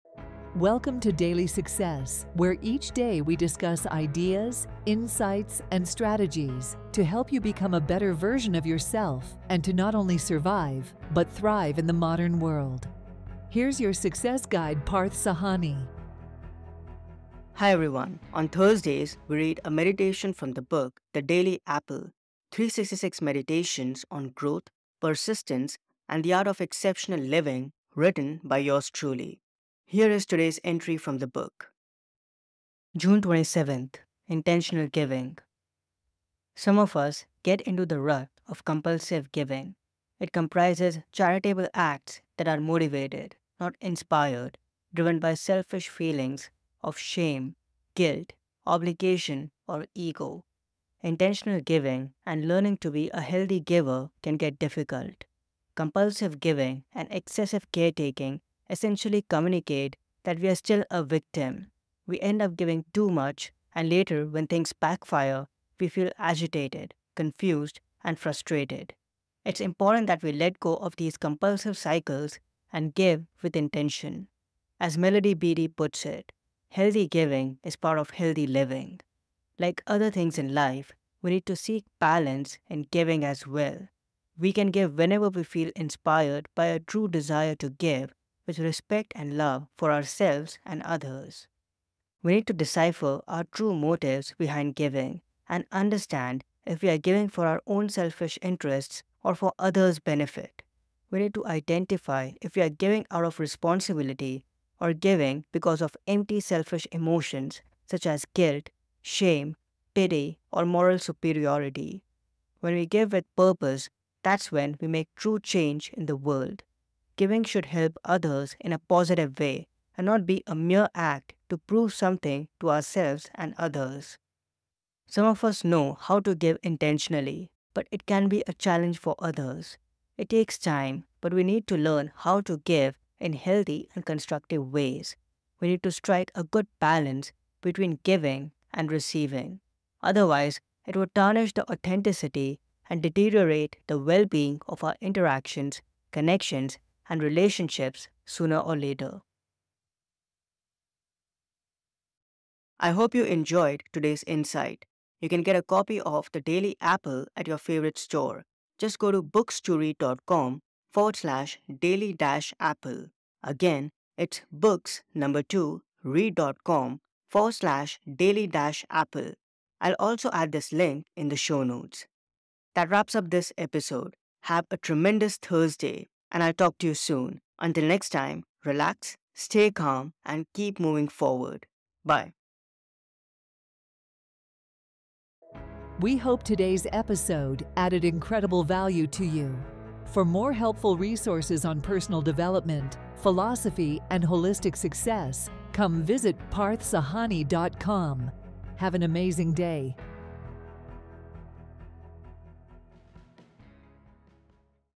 Intentional Giving | An Apple a Day
Some of us know how to give intentionally, but it can be a challenge for others. It takes time but we need to learn how to give in healthy and constructive ways. We need to strike a good balance between giving and receiving. Otherwise, it would tarnish the authenticity and deteriorate the well-being of our interactions, connections, and relationships sooner or later.